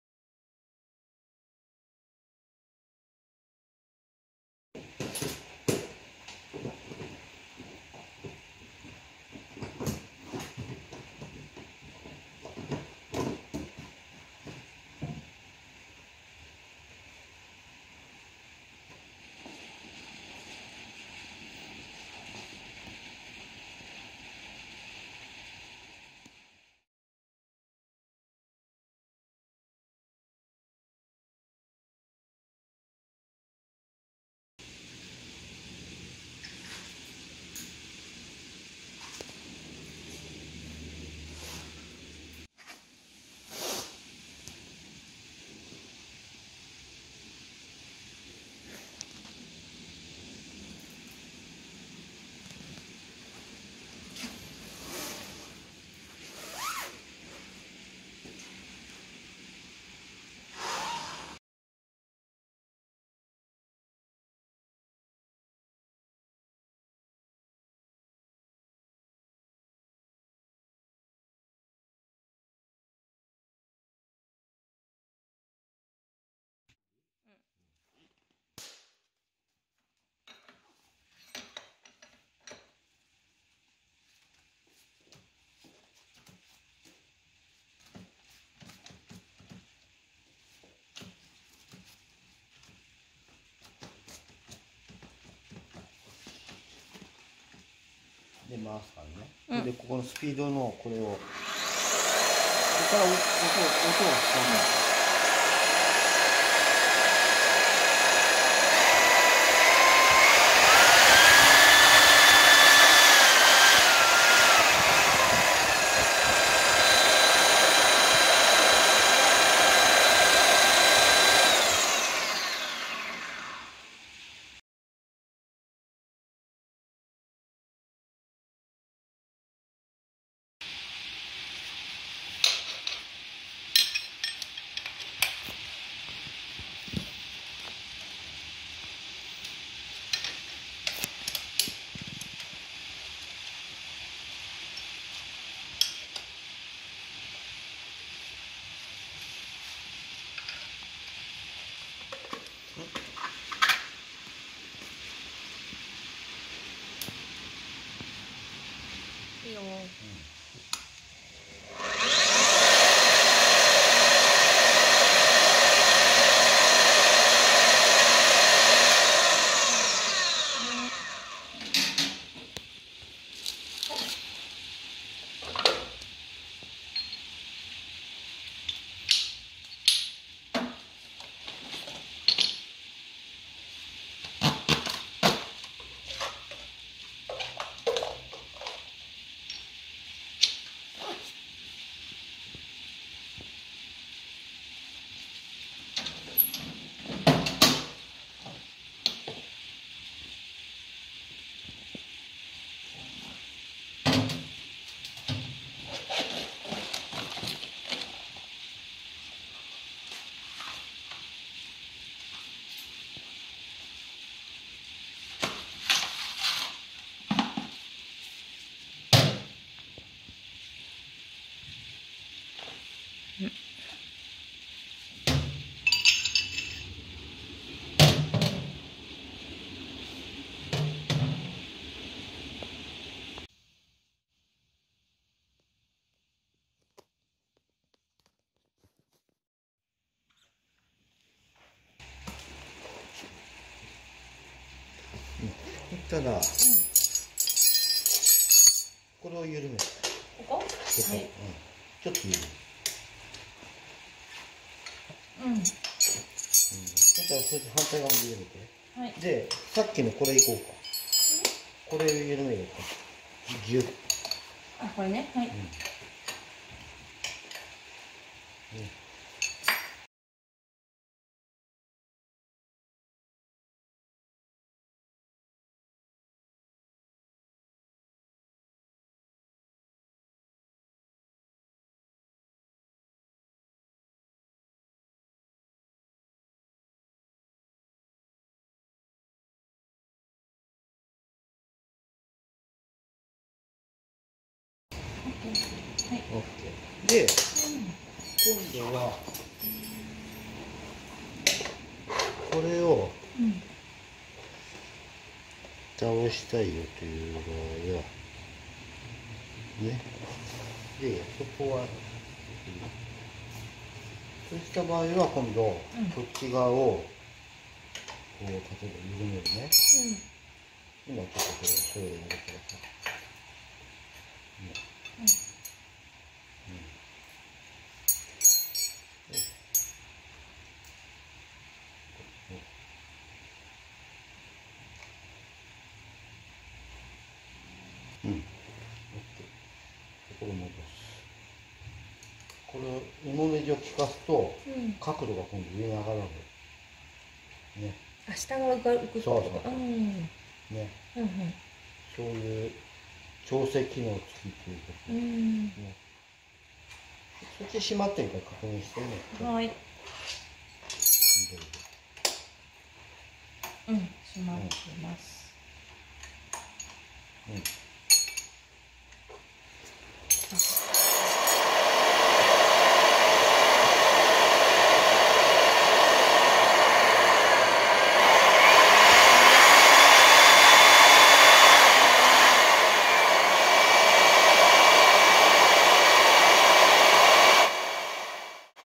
Customized series① MAB 1300V with added adjustment function
Introducing the MAB1300V German-made magnetic core drill machine specifications.
We have mounted an original tray which provides fine adjustment functions.
See it tested out in actual operations.

We also have other videos available for viewing, so feel free to check them out.